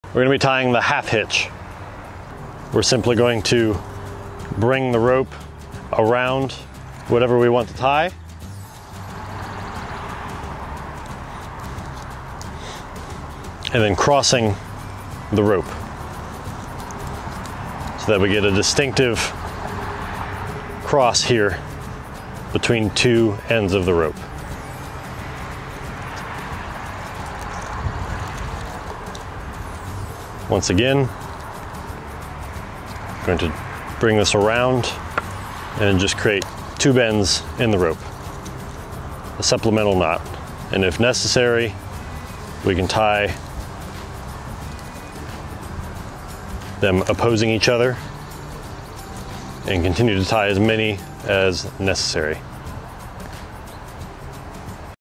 How to tie a half hitch
How to tie a half hitch by ISA Certified Arborist Jacob Nau, our training and development manager!